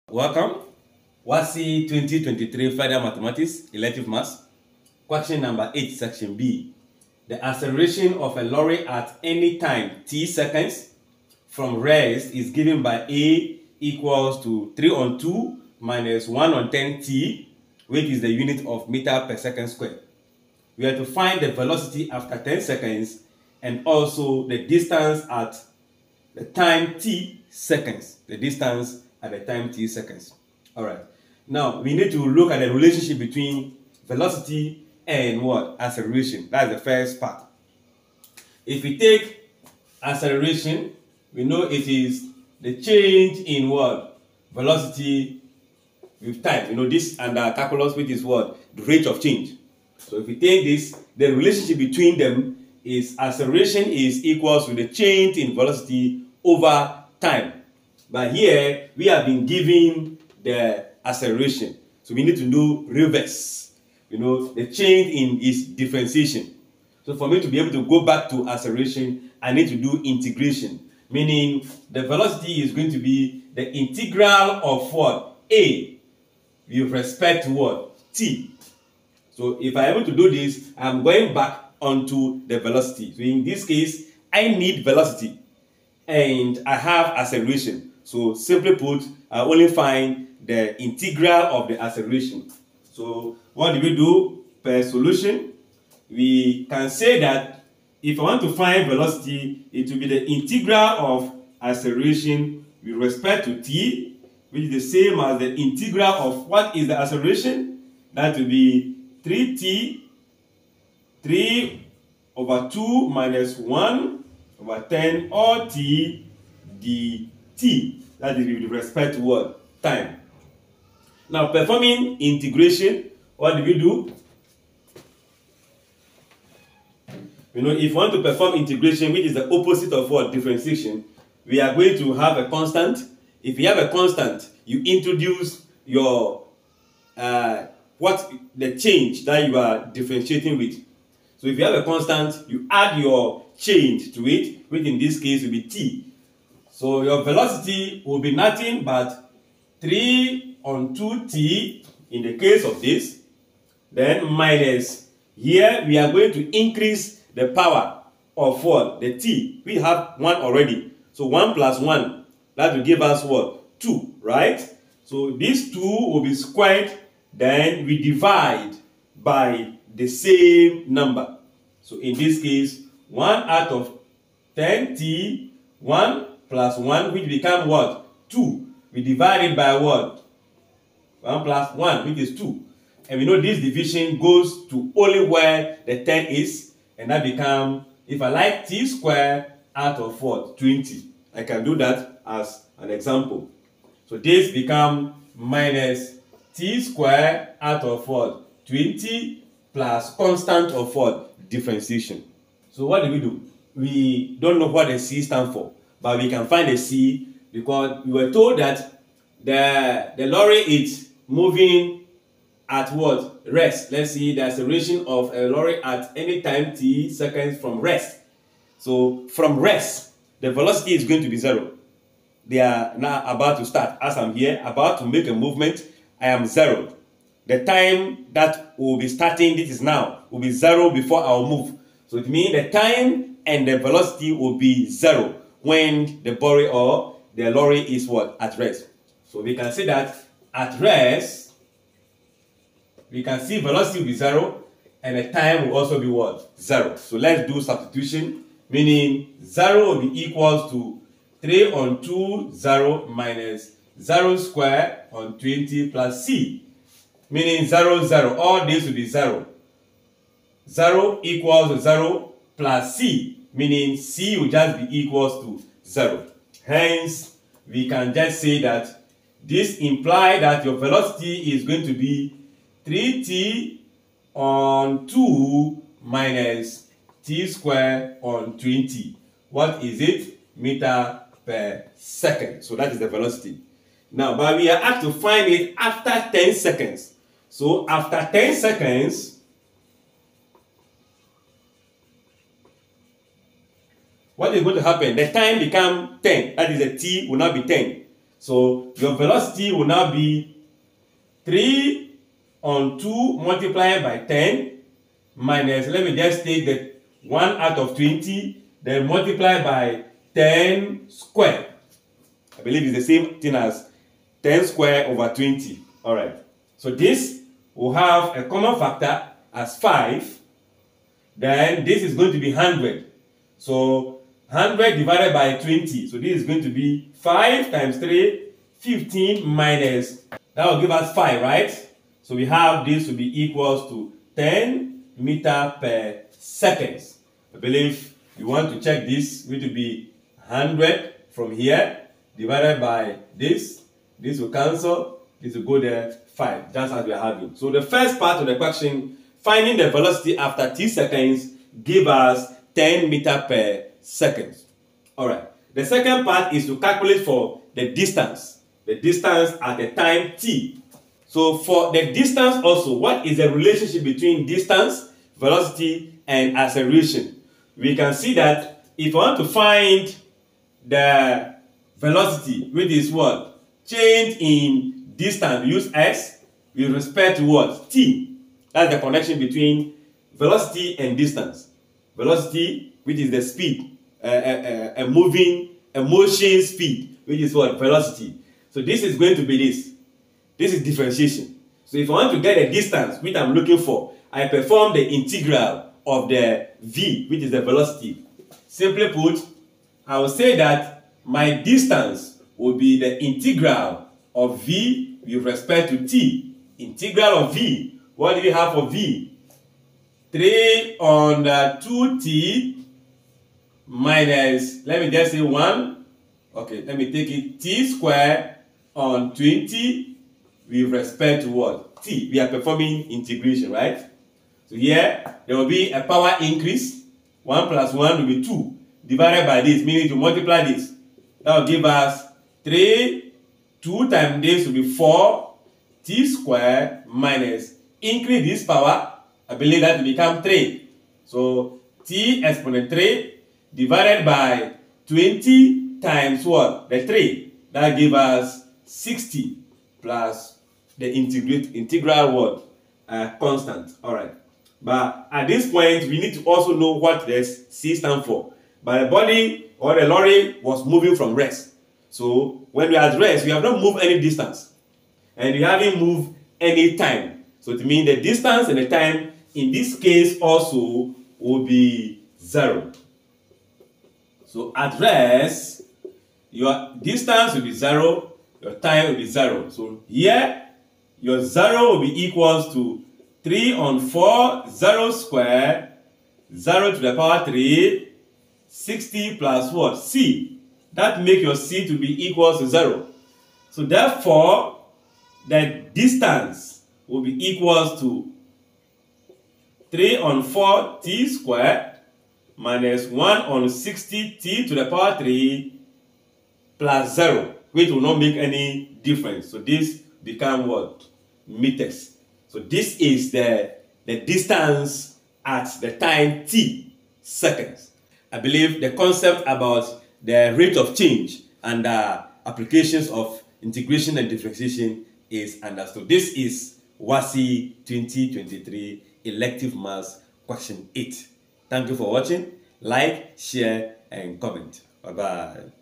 WASSCE 2023 FURTHER MATHEMATICS QUESTION No. 8 ON RATE OF CHANGE (INTEGRATION)
Solving wassce further mathematics question on rate of change